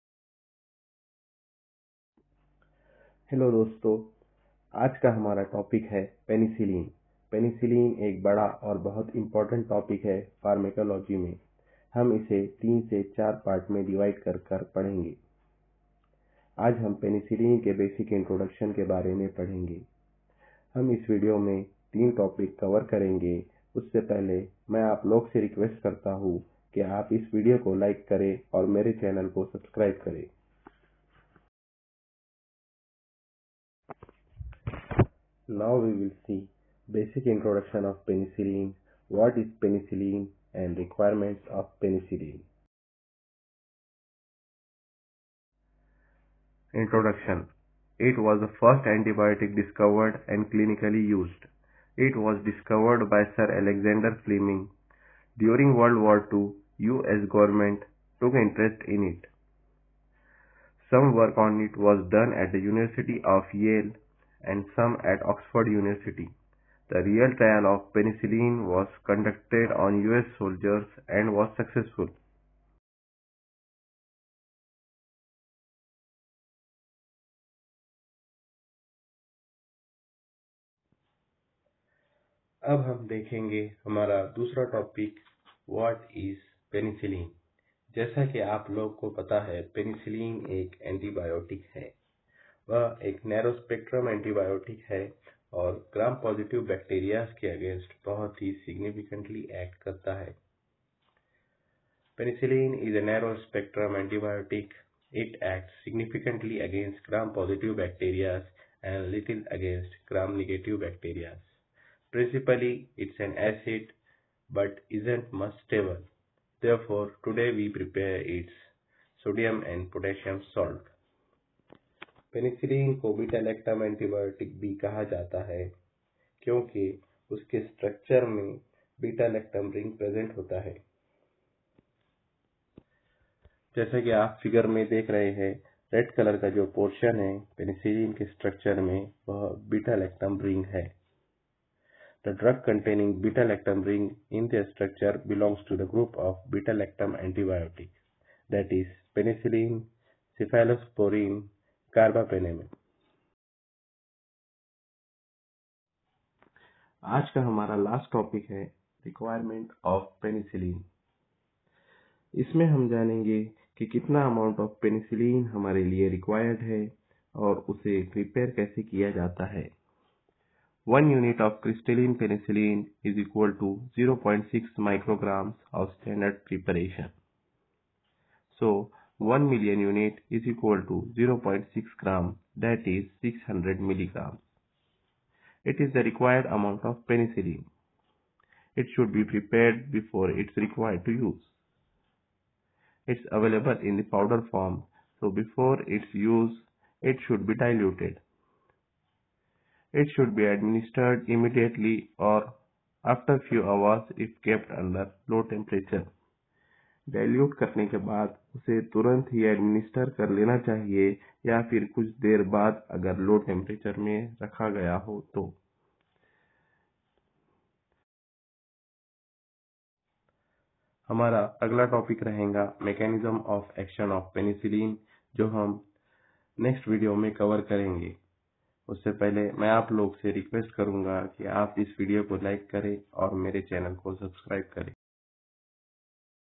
Penicillin | Introduction | Pharmacology | Topic no:- 1 | Hindi / English | Simple Language
Hello friends,
learn pharmacology in very simple language in hindi
1st video on basic introduction of penicillin